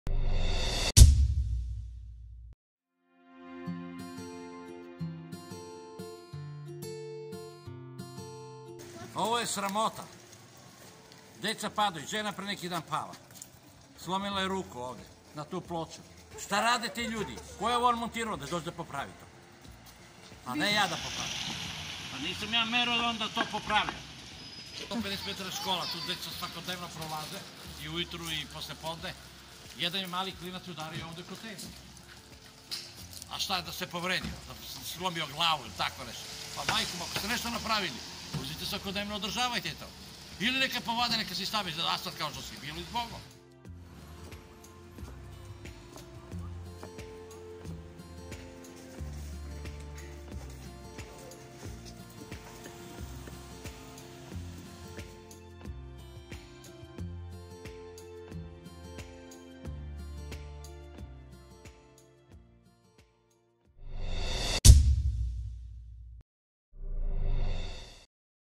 "Popustile" behaton ploče na trotoaru, dodatna opasnost po snegu i ledu
"Kad padne sneg igramo slalom na trotoaru i pazimo da ne polomimo ruku, nogu, glavu od ovih behaton ploča“, tako Nišlije opisuju kretanje u ulici Todora Milovanovića, na prostoru između Doma zdravlja i Voždove škole. Ploče koje se klate, podignute i nestabilne posebno su rizične za osobe sa invaliditetom. Klate se i one koje bi slepima i slabovidima trebalo da omoguće bezbedno kretanje.